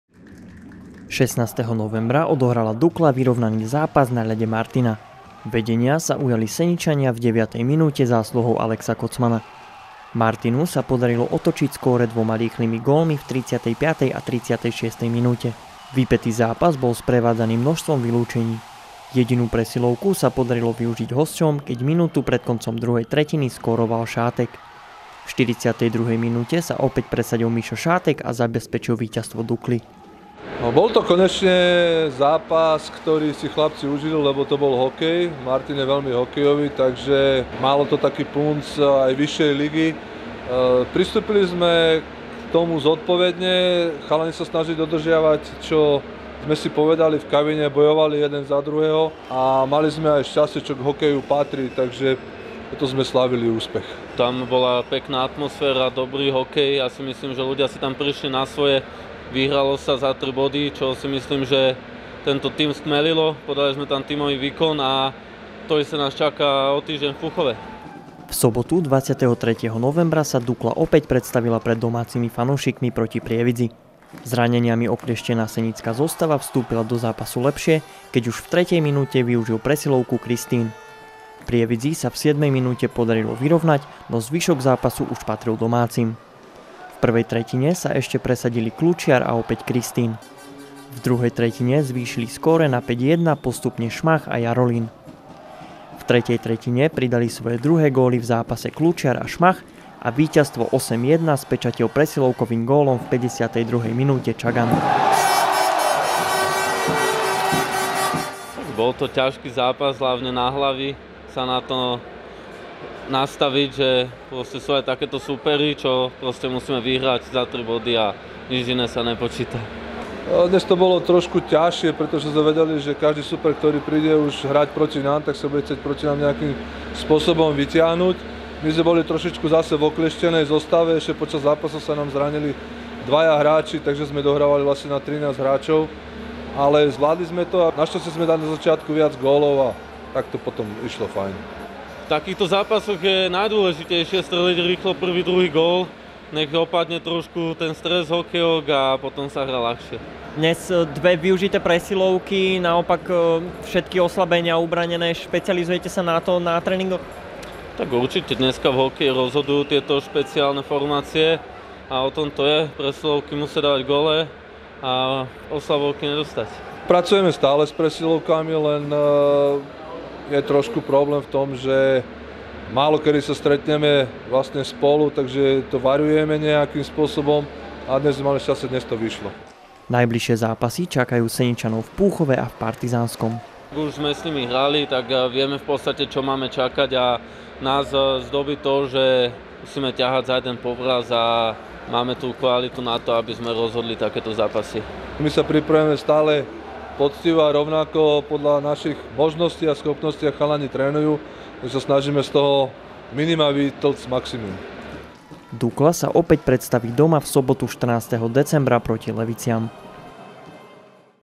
Dukla zdolala Martin i Prievidzu
Dukla obhájila prvenstvo v tabuľke po výhre 8:1 proti Prievidzi